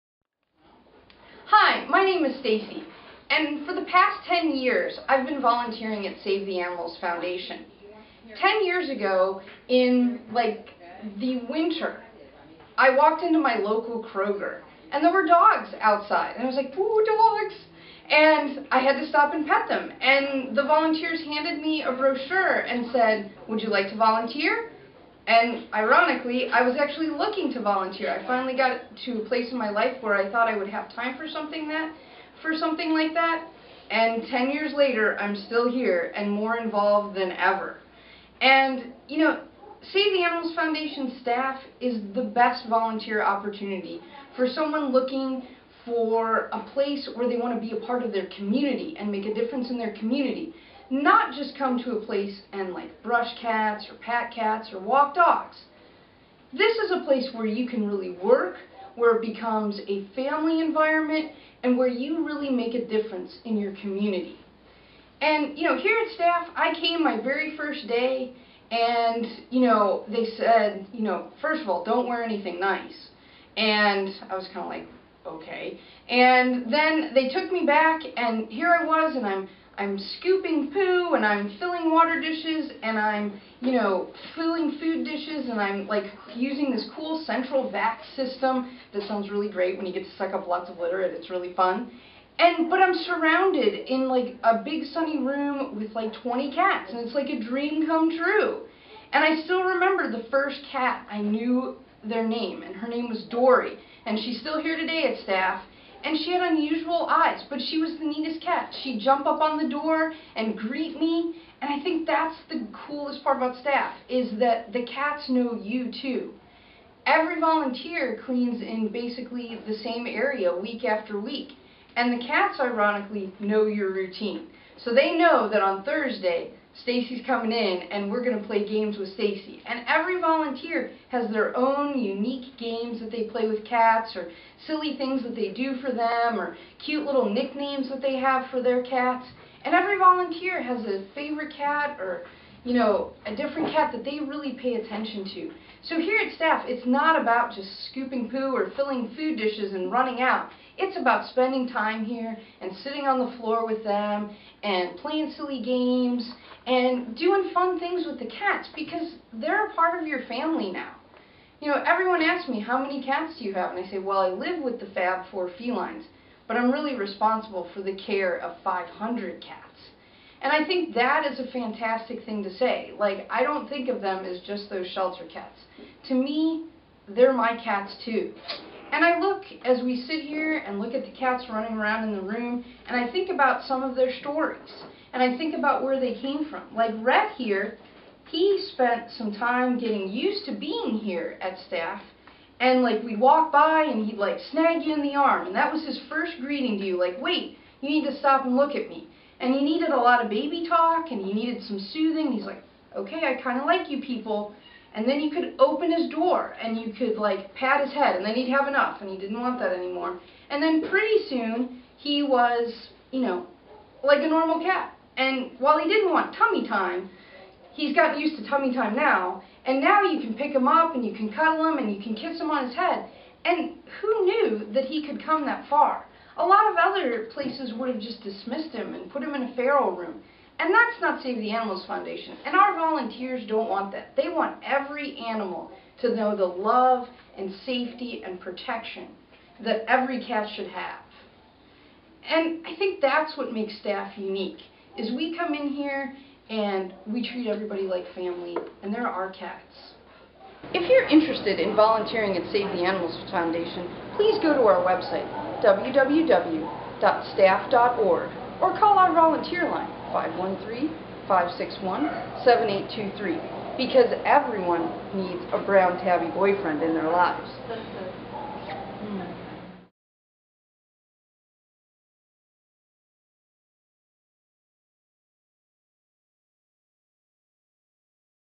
Volunteer at Save the Animals Foundation
Watch as Stacy shares her volunteer story among a brightly lit room of frolicking cats. Save the Animals Foundation (STAF) is always open to new volunteers with either cats or dogs.